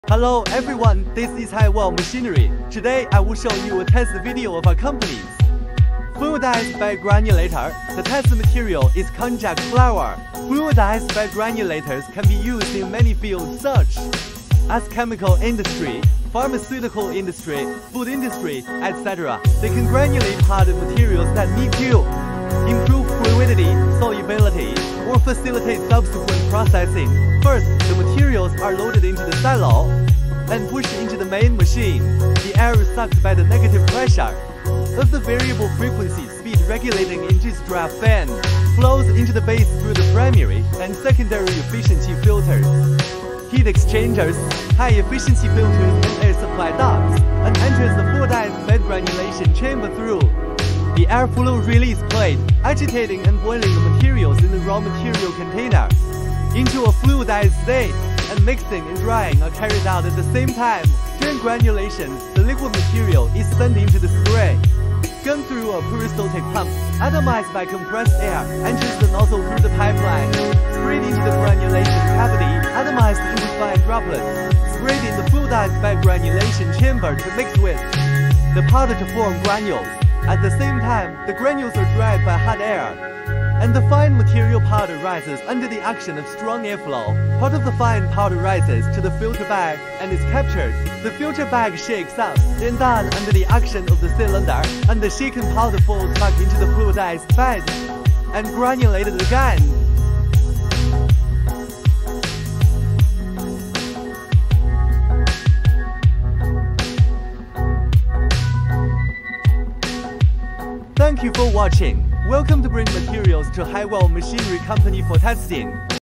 Fluidized Bed Granulator for Fiber Drink Instant Granules
Today I will show you a test video of our company's fluidized bed granulator. The test material is konjac flour. Fluidized bed granulators can granulate powdered materials that need to improve fluidity, and solubility, or facilitate subsequent processing. The air is sucked by the negative pressure of the variable frequency speed regulating induced draft fan, flows into the base through the primary and secondary efficiency filters, heat exchangers, high-efficiency filters, and air supply ducts, and enters the fluidized bed granulation chamber through the air flow release plate, agitating and boiling the materials in the raw material container into a fluidized state, and mixing and drying are carried out at the same time.